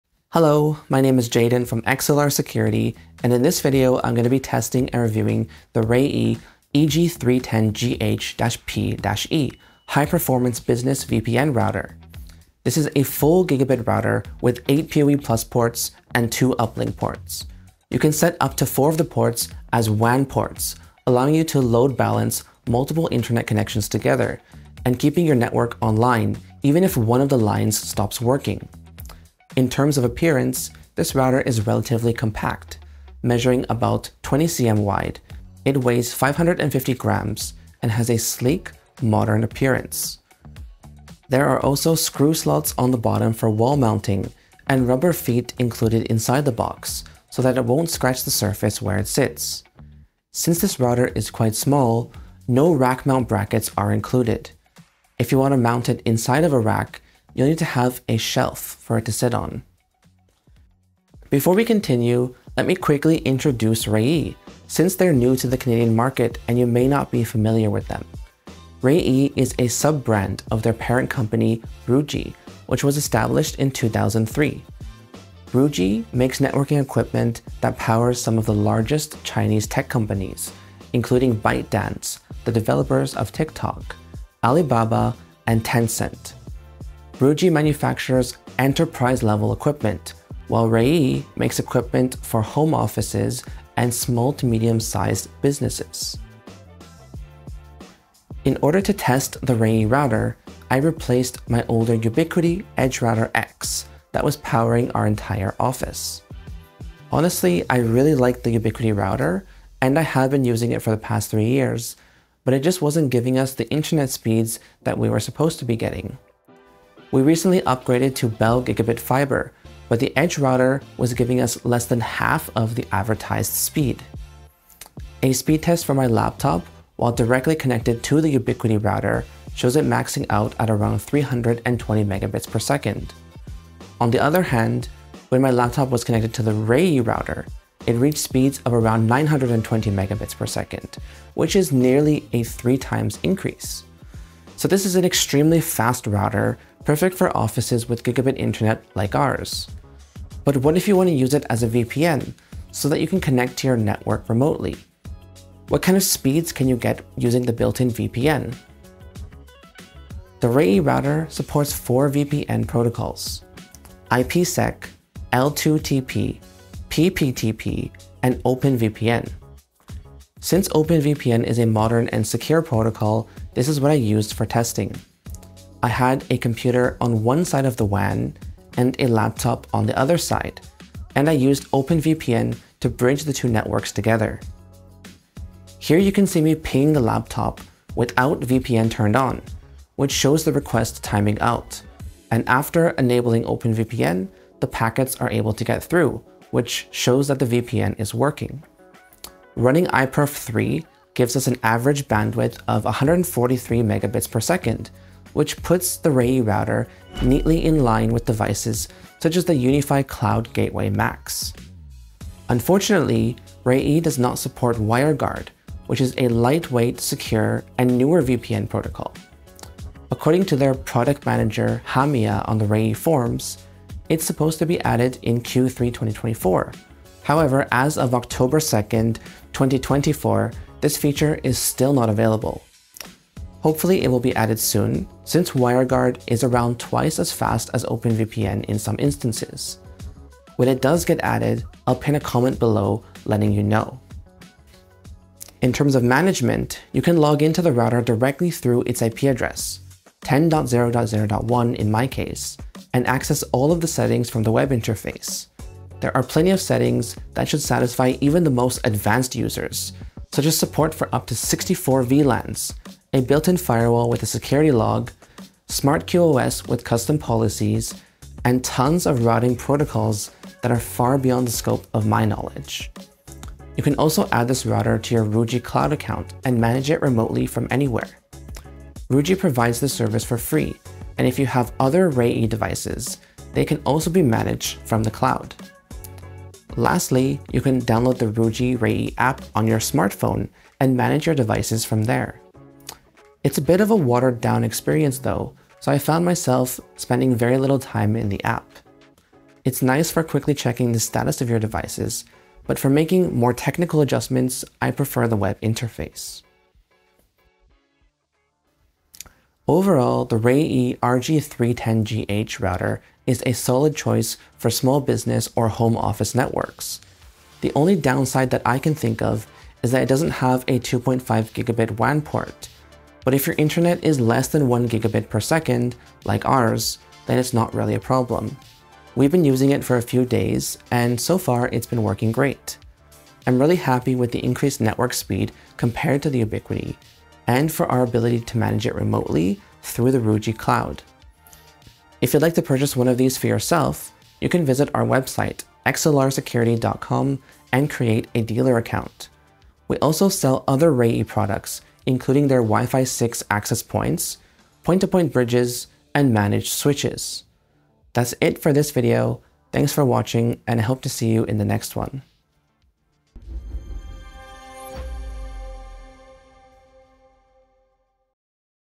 The Perfect Router for Small Business? Reyee EG310GH-P-E | Review & Features
The Reyee EG310GH-P-E is a great choice for small businesses that want fast network speeds, a built-in VPN, multiple WAN ports, and VLAN functionality. This router also has 8 PoE ports which supply up to 30 W of power for security cameras, IP phones, or wireless APs.

You can login to the router locally using the IP address, or remotely using the free Ruijie cloud portal. You'll have to register for an account to use the cloud portal, but it's worth the convenience of logging into your router from anywhere. You can configure VLANs, issue VPN certificates, and view network traffic without being physically on site.

*Timestamps*
0:00 – Features & Appearance
1:00 – Who is Reyee?
1:37 – How Fast the Reyee Router?
2:40 – VPN Support & Speeds
4:05 – Cloud Management
5:08 – Should You Get It?